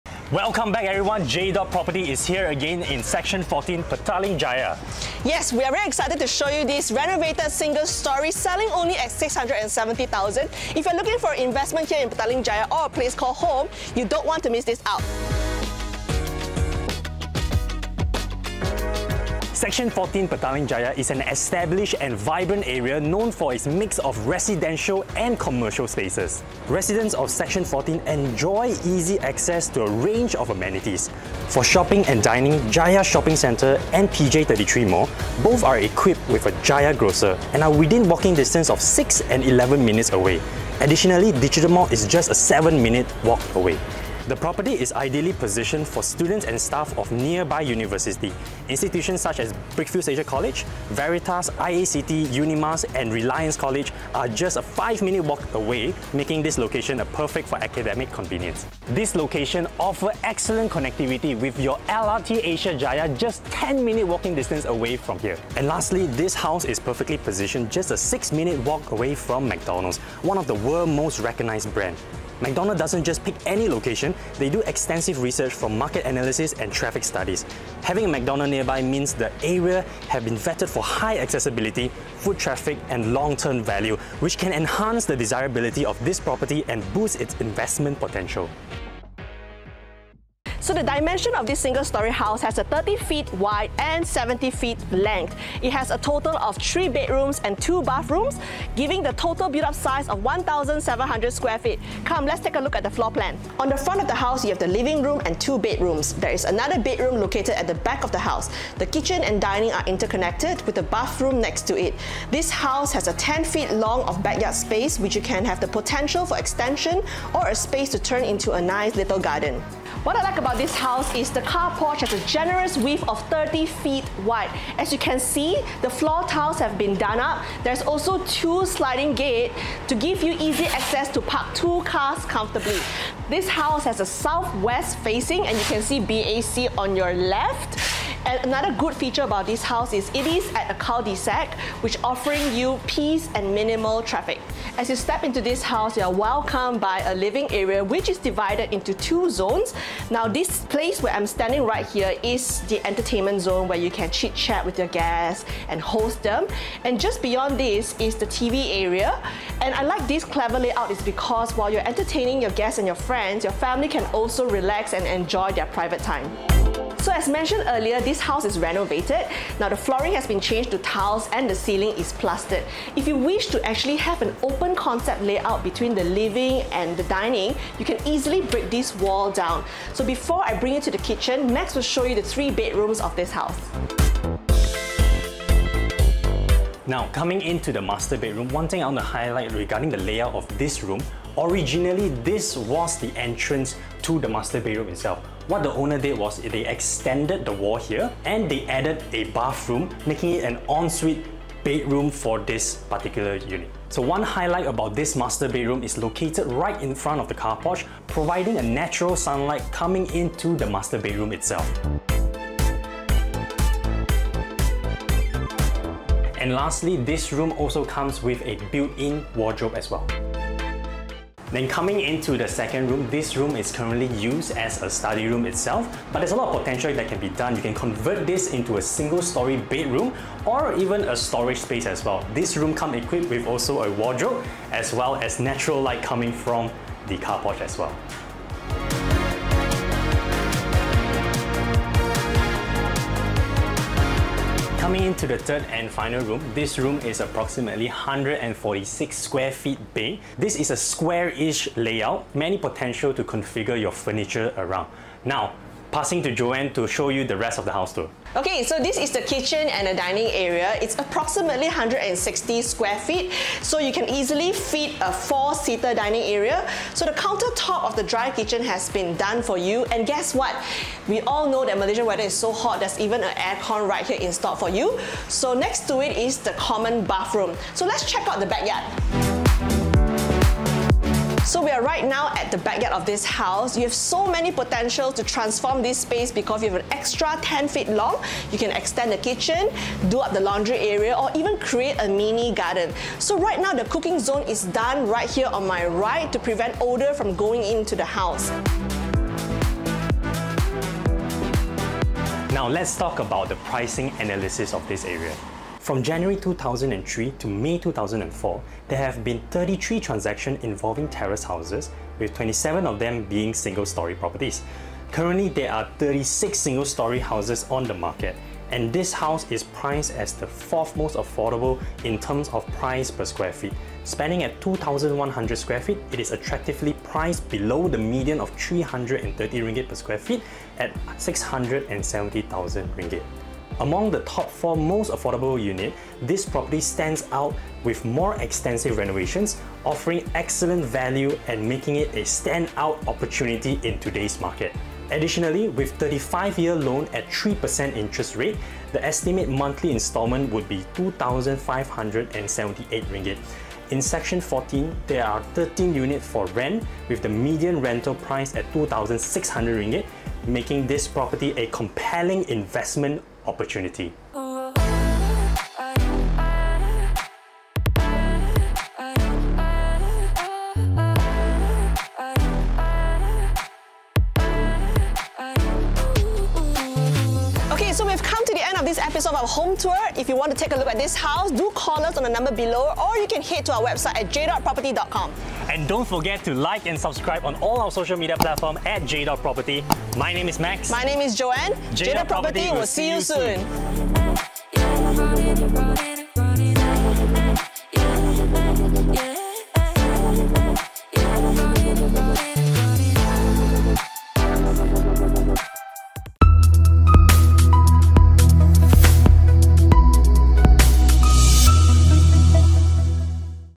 Seksyen 14, Petaling Jaya | RM 670,000 | Single-Storey Terrace House | Jdot Property
Join Joanne and Max on this home tour to check out this renovated single-storey house in Seksyen 14 (Section 14) of Petaling Jaya.

This 1,700 sqft built-up home on a 2,100 sqft (30' x 70') lot features 3 bedrooms, 2 bathrooms, and parking for 2 cars. It is partially furnished and has been owner-occupied, keeping the property in good condition.

Located on a quiet cul-de-sac street, it is just a short walk to BAC University, Jaya 33, Jaya Shopping Mall, Digital Mall, and Asia Jaya LRT. Renovated 13 years ago with approvals, this South-West facing property offers a great location with both convenience and comfort.

Timestamp:
0:00 Introduction
0:19 Location & Amenities
1:30 Property Facts
1:42 Floor Plan
2:00 Car Porch
2:25 Living Area
3:04 Bedrooms
4:18 Dining Area & Kitchen
4:39 Backyard
5:02 Price Analysis in Seksyen 14